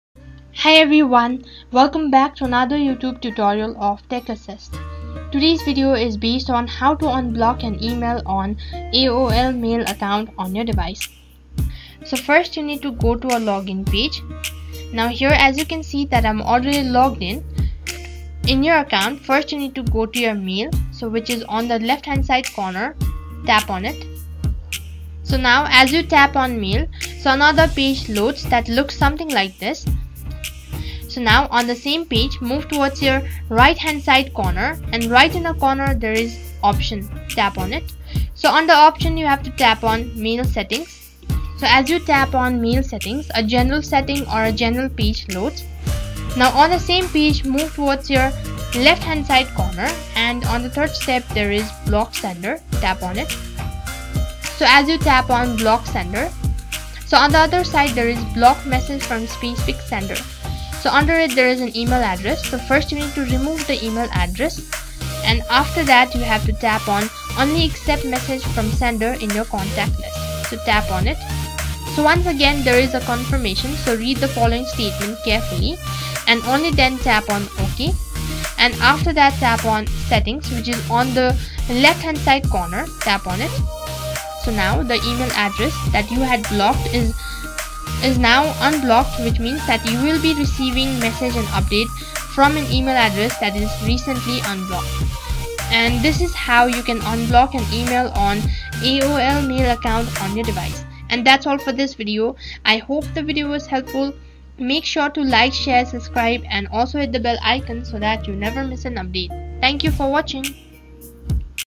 How to Unblock an Email on AOL Mail
This video is based on how to unblock an email on AOL mail account.